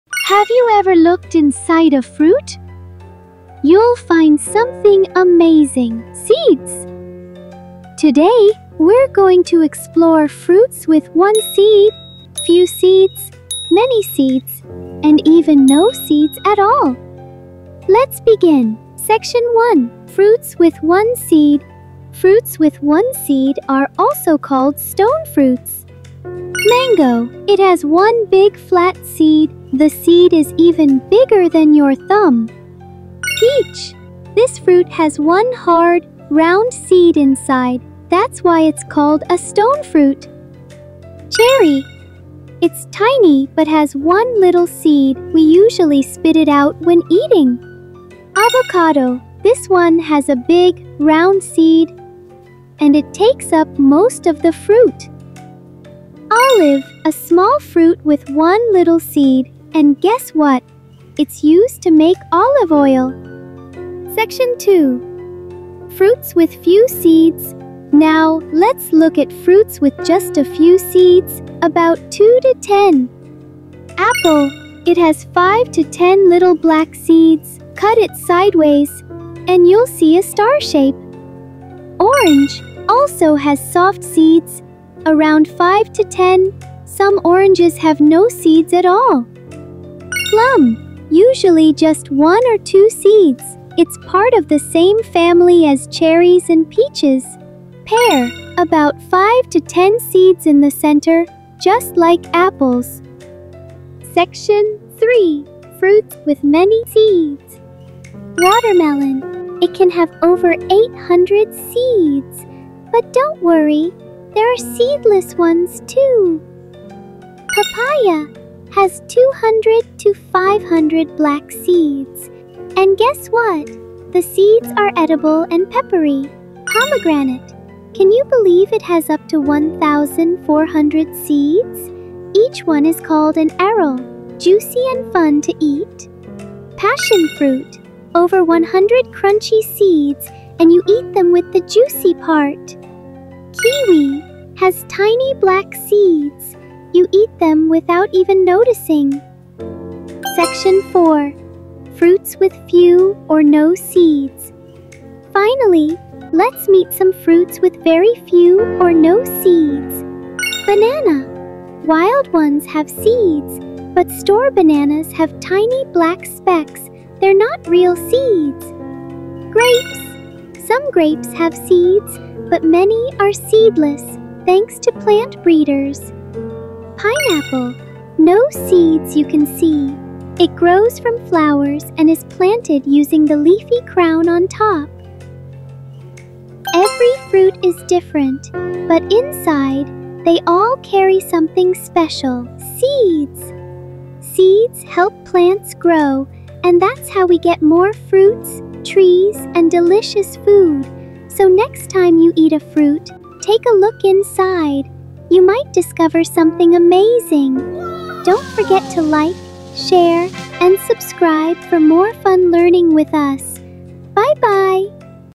Fruits and Their Seeds 🌱 | One Seed, Few Seeds, Many & No Seeds | Fun Educational Video for Kids
🌈 Discover What’s Inside Fruits — SEEDS! | Fun & Educational Fruit Seed Video for Kids 🌱🍎

Have you ever looked inside a fruit?
You’ll find something amazing… Seeds!

In this colorful and fun educational video, we explore all kinds of fruits based on how many seeds they have — from fruits with one seed (like mango and avocado), to fruits with few seeds (like apples and oranges), to fruits with LOTS of seeds (like watermelon and pomegranate), and even fruits with NO visible seeds (like bananas and grapes)!

👀 Watch REAL fruit being sliced open with clear close-ups of seeds inside.
🎨 Every fruit is shown in vivid, original footage with bright colors and real textures.

📚 What You’ll Learn in This Video:
 • What are stone fruits (fruits with ONE seed)?
 • Which fruits have a few seeds inside?
 • Which fruits are packed with hundreds of seeds?
 • Can some fruits have no seeds at all?

🧒 Perfect for:
 • Kids learning about fruits and plants 🌱
 • Preschool and elementary science education 🍎
 • Parents and teachers who want fun learning content 🎓
 • Curious minds who love exploring food and nature! 🥭🍇🍍